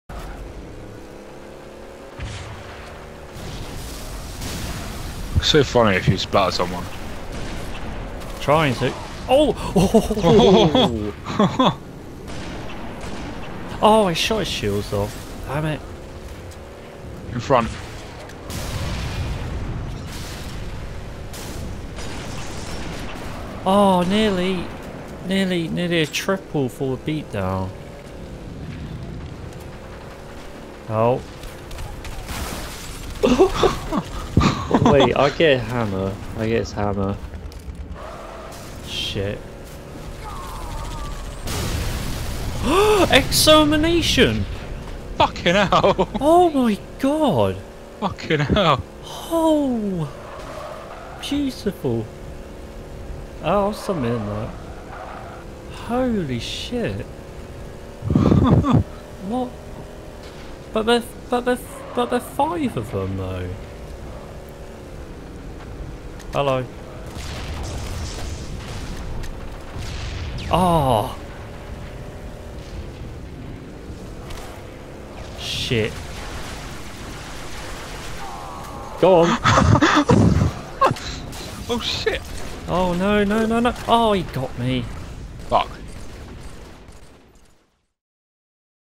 The Mongoose Grind - Ratnest Madness
finally moved house so played a few hours with a ok-ish setup and got this

Also now I finally got better internet to upload and play without getting disconnected